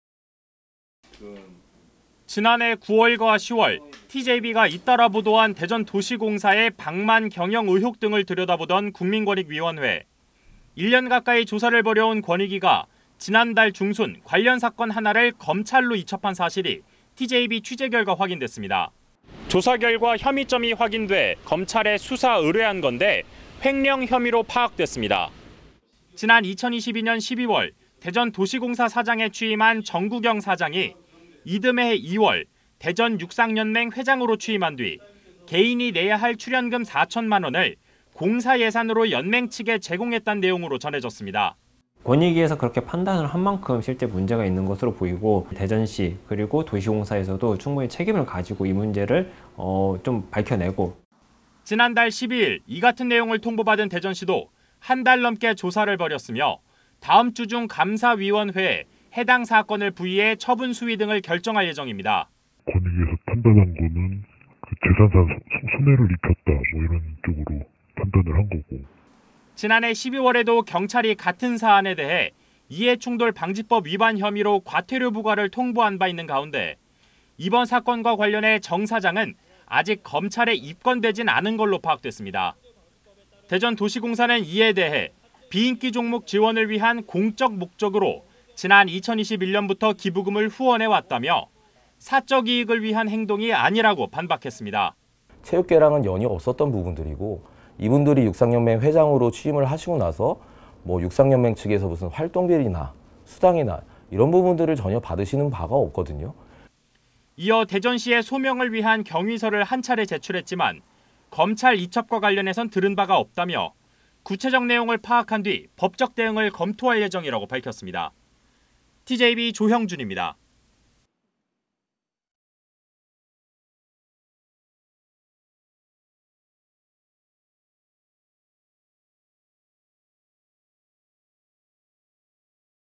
권익위, 대전도시공사 사장 검찰 수사 의뢰…"횡령 혐의" / SBS /
지난해 9월과 10월 TJB가 잇따라 보도한 대전도시공사의 방만 경영 의혹 등을 들여다보던 국민권익위원회.

1년 가까이 조사를 벌여온 권익위가 지난달 중순 관련 사건 하나를 검찰로 이첩한 사실이 TJB 취재 결과 확인됐습니다.

조사 결과 혐의점이 확인돼 검찰에 수사 의뢰한 건데 횡령 혐의로 파악됐습니다.

지난 2022년 12월 대전도시공사 사장에 취임한 정국영 사장이 이듬 해 2월 대전육상연맹 회장으로 취임한 뒤, 개인이 내야 할 출연금 4천만 원을 공사 예산으로 연맹 측에 제공했다는 내용으로 전해졌습니다.

[설재균/대전참여자치시민연대 의정감시팀장 : 권익위에서 그렇게 판단을 한 만큼 실제 문제가 있는 걸로 보이고, 대전시 그리고 도시공사에서도 충분히 책임을 가지고 이 문제를 밝혀내고…]

지난달 12일 이 같은 내용을 통보받은 대전시도 한 달 넘게 조사를 벌였으며, 다음 주 중 감사위원회에 해당 사건을 부의해 처분 수위 등을 결정할 예정입니다.

[대전시 관계자 : 권익위에서 판단한 거는 '(공사에) 재산상 손해를 입혔다', 이런 쪽으로 판단을 한 거고…]

지난해 12월에도 경찰이 같은 사안에 대해 이해충돌방지법 위반 혐의로 과태료 부과를 통보한 바 있는 가운데, 이번 사건과 관련해 정 사장은 아직 검찰에 입건되진 않은 걸로 파악됐습니다.

대전도시공사는 이에 대해 비인기 종목 지원을 위한 공적 목적으로 지난 2021년부터 기부금을 후원해왔다며 사적 이익을 위한 행동이 아니라고 반박했습니다.

[유환진/대전도시공사 법무팀장 : 체육계랑은 연이 없었던 분들이고 이분들이 육상연맹 회장으로 취임하고 나서 육상연맹 측에서 무슨 활동비나 수당이나 이런 부분들을 전혀 받는 바가 없거든요.]

이어 대전시에 소명을 위한 경위서를 한 차례 제출했지만, 검찰 이첩과 관련해선 들은 바가 없다며 구체적 내용을 파악한 뒤 법적 대응을 검토할 예정이라고 밝혔습니다.

(취재 : 조형준 TJB, 영상취재 : 송창건 TJB, 영상편집 : 원형희, 제작 : 디지털뉴스편집부)